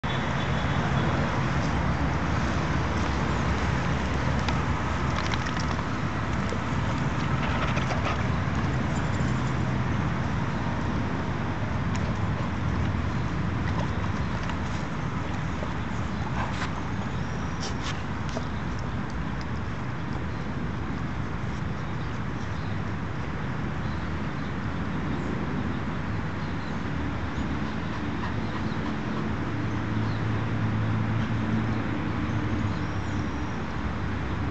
Lobinho, o Pastor Tibetato (ele curte uma Lama!)
Lobinho e Belinha, na praça Amália Zen Cavinato, na Vila Gonçalves, São Bernardo do Campo, SP. Em 28/11/2015.